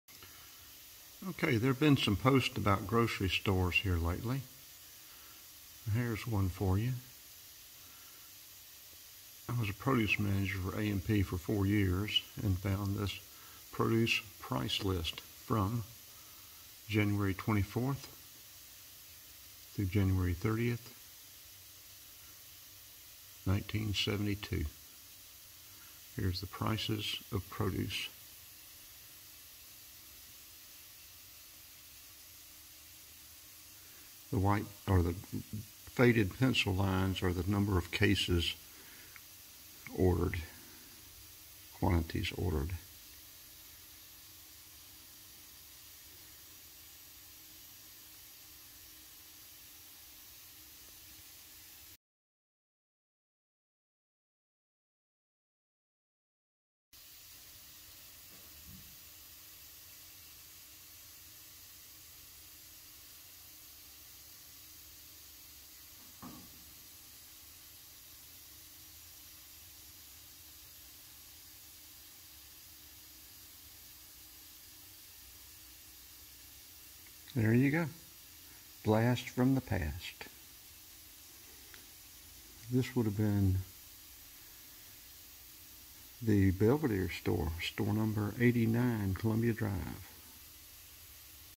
A & P Tea Company Columbia Drive Store October 27th 2021
Produce prices from A&P Store #89 Columbia Drive Decatur Ga January 1972 almost 50 years ago.